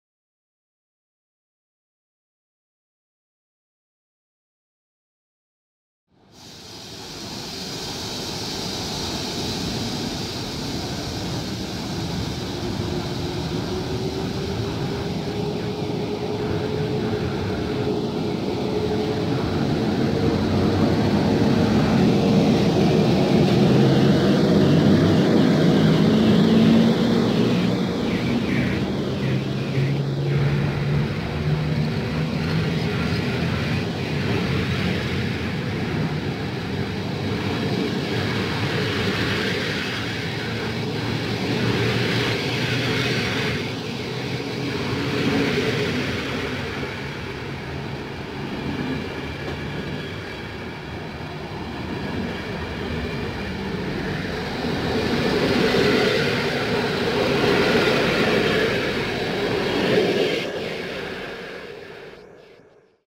EP09-010 Opocznianka z IC 5354/5 Artus relacji Hel - Przemyśl Główny odjeżdża ze Żmigrodu.
EP09-010 Opocznianka z IC 5354/5 Artus relacji Hel - Przemyśl Główny o długości 1074 kilometrów odjeżdża opóźniony godzinę ze stacji Żmigród w kierunku Wrocławia.

Województwo: Dolnośląskie
Miejscowość: Żmigród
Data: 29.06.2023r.
Godzina: 16:05
Przewoźnik: PKP Intercity
D29: 271 (Wrocław - Poznań)
Montaż: Camtasia Studio